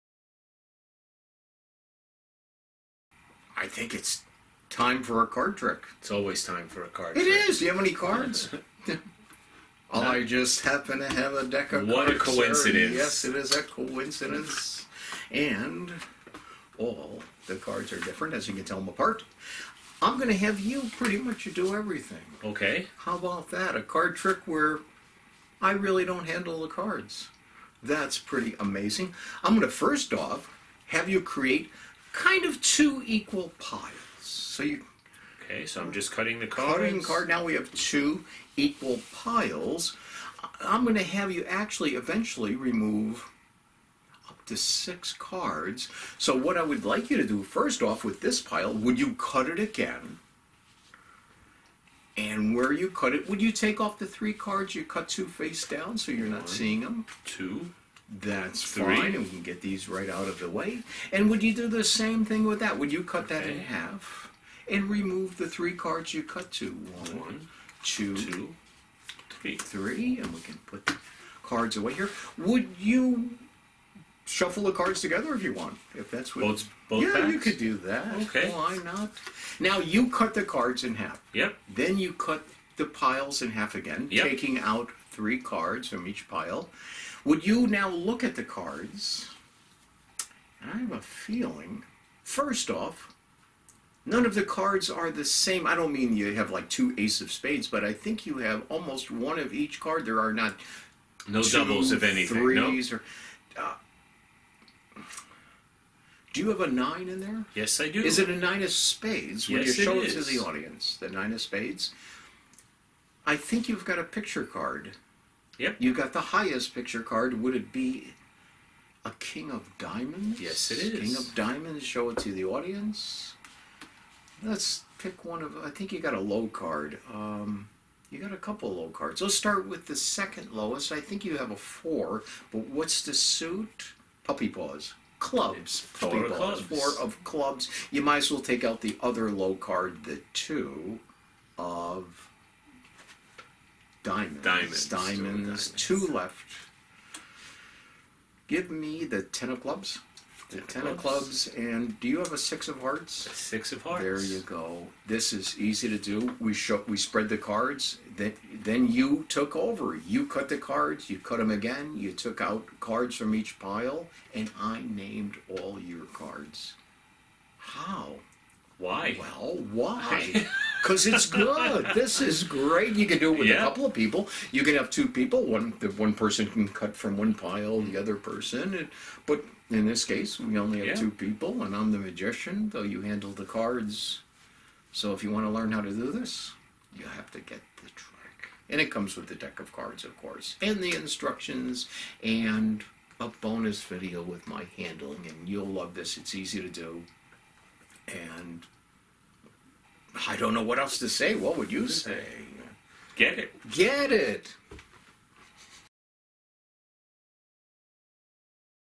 Six Card Sensation Is a BEST SELLER! - MagicTricks.com
: Multiple piles, all in their hands- and they match!

Very entertaining match- all in the control of your spectator!

All we can say is- ABSOLUTELY AMAZING!
Everything seems to be under the control of your spectator, but the magic miracle still happens!

You see the routine in the demo video above.
Sideshow Steve was the spectator, and he FREELY chose his six cards in a random way from the deck.
Yet Peter was instantly able to correctly name ALL SIX CARDS chosen!

What a clever method!
You will be able to perform this with a minimum of practice.
Stun your friends!